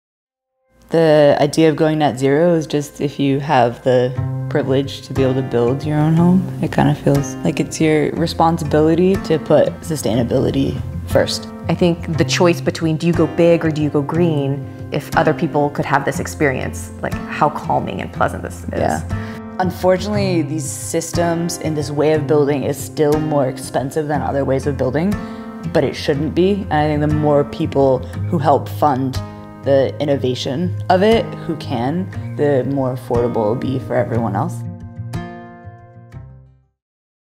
Go Zero Tour 2022 SHORTS - Richmond Residence
Go Zero Tour presented by Energy Trust of Oregon is Oregon's largest annual zero energy homes tour. This Oct 8 & 9, step inside eight "net zero" homes in Portland and learn about how you can go zero. Tickets and info: